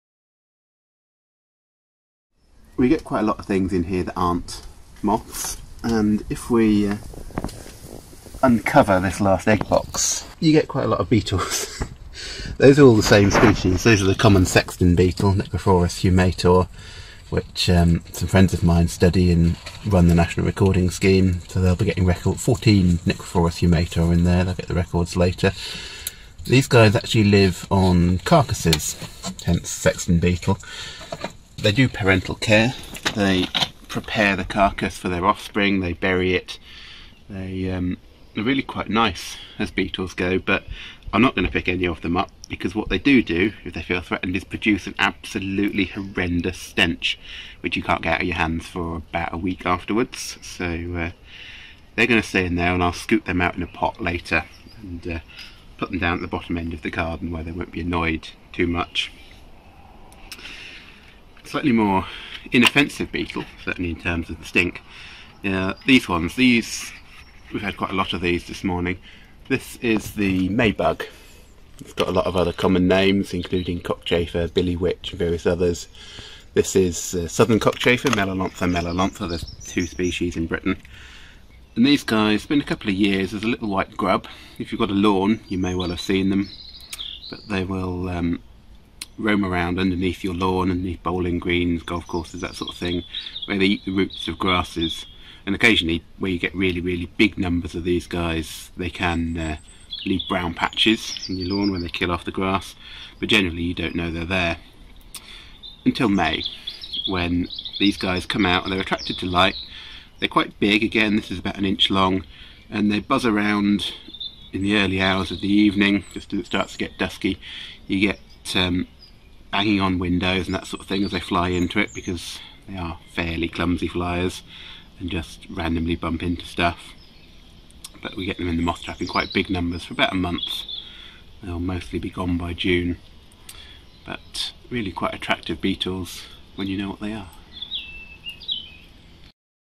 Light trap beetles - Sexton beetle and Maybug
It's not just moths that come to light - plenty of beetle species do as well. Here we have the Common Sexton beetle Nicrophorus humator and the Maybug (aka Cockchafer) in the garden moth trap